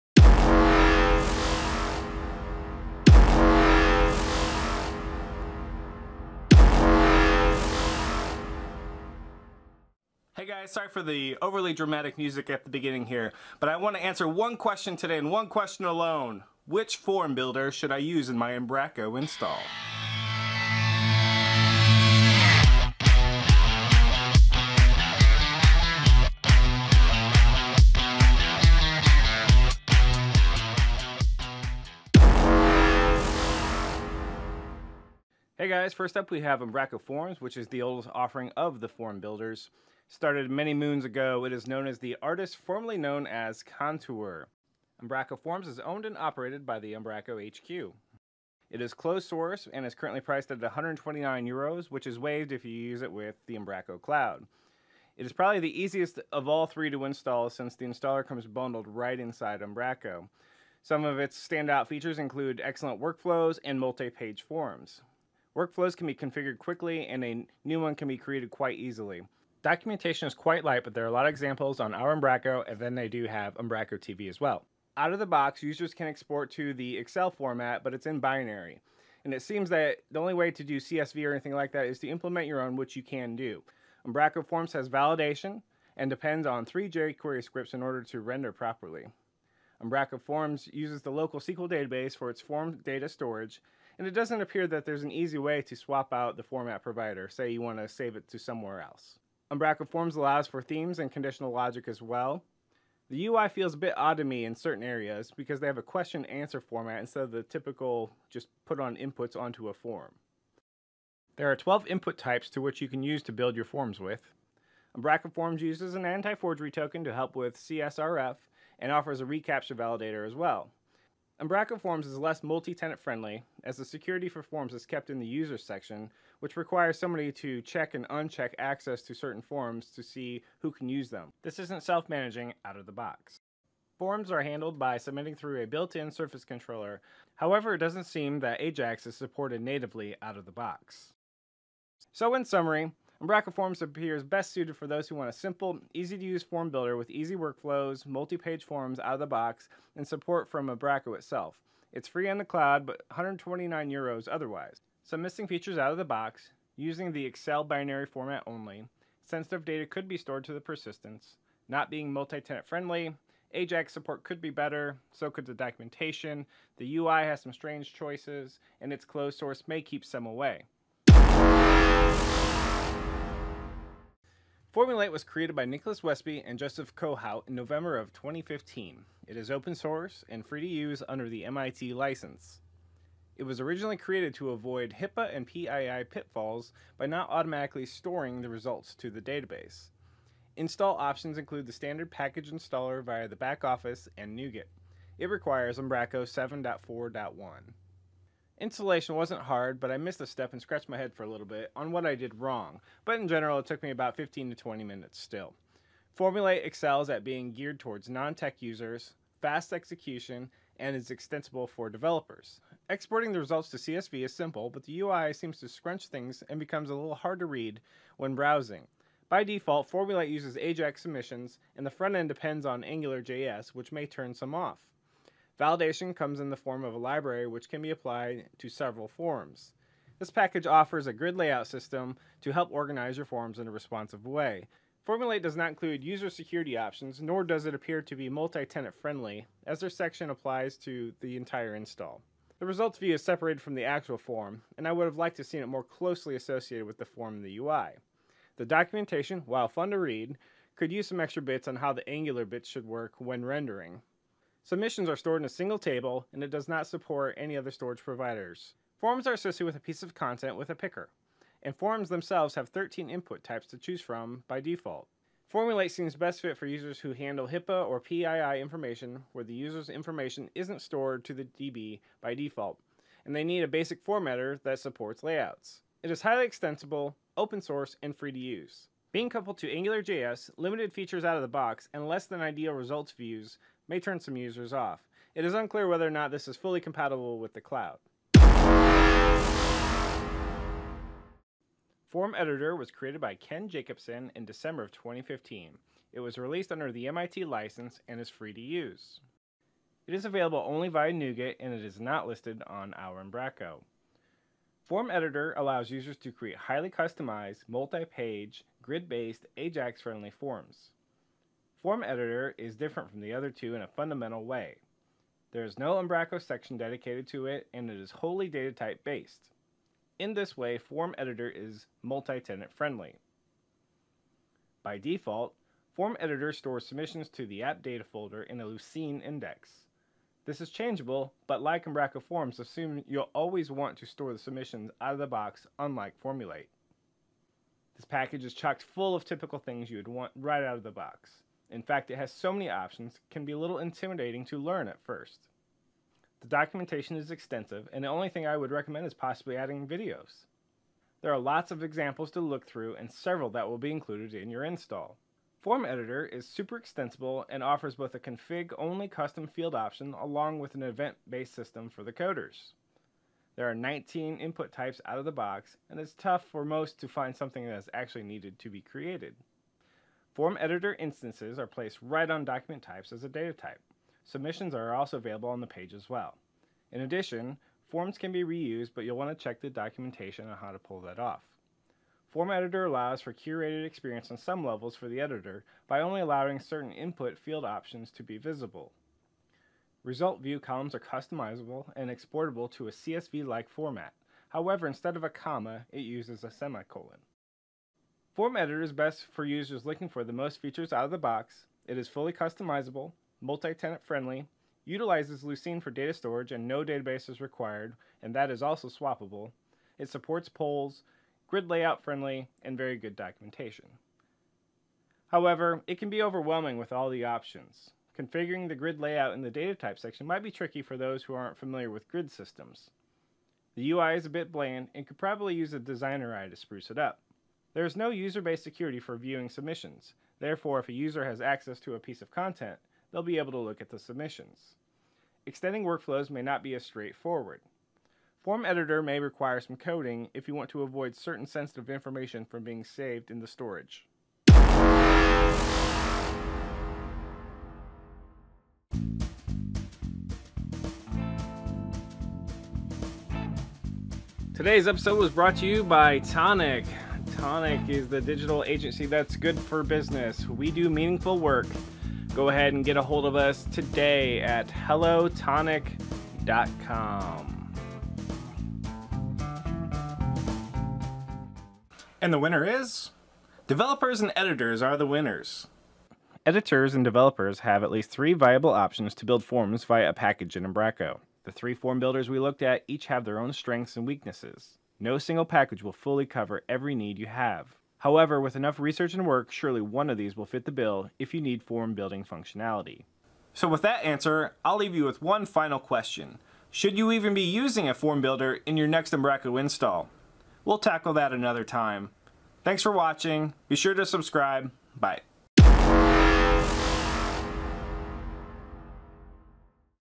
Umbraco Forms vs Formulate vs FormEditor
I take a quick look at Umbraco Forms, Formulate and FormEditor available for the Umbraco CMS.

Umbraco Forms: Included in your install already!